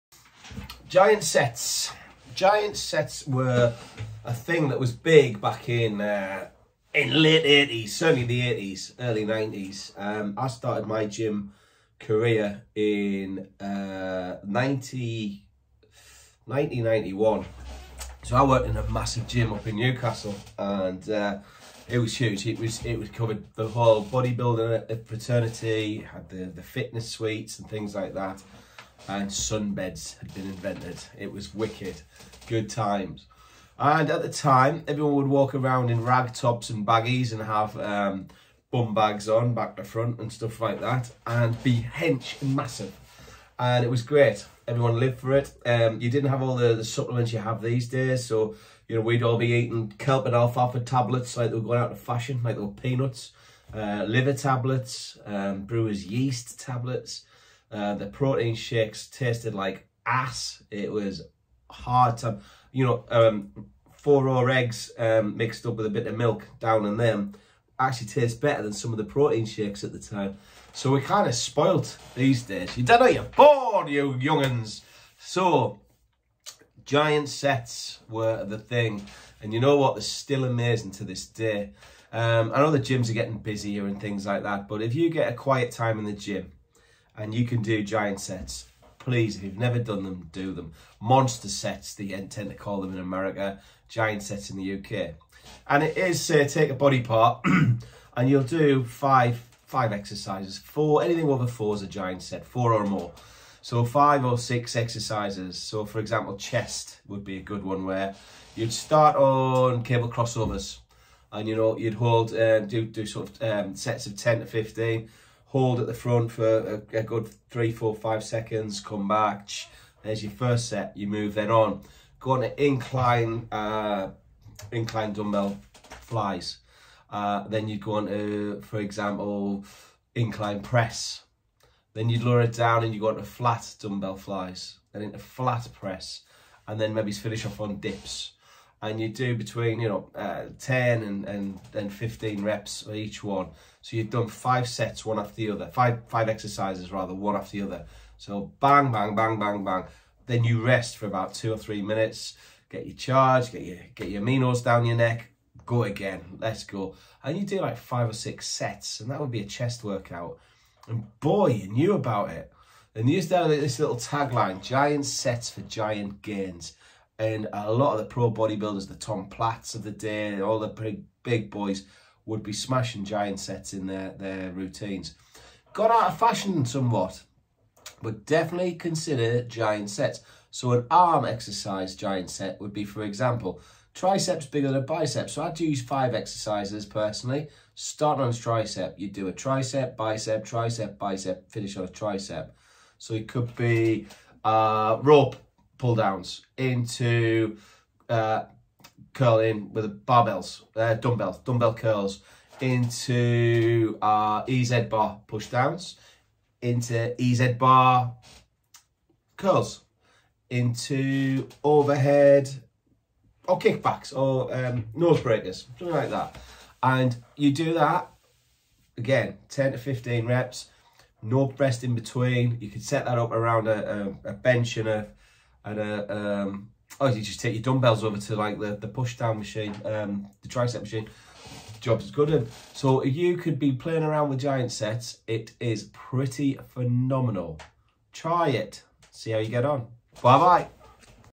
Giant Sets for Giant Gains!
people get stuck in a rut when it comes to training and don't change things up enough. here is a great little training method to stimulate muscular development.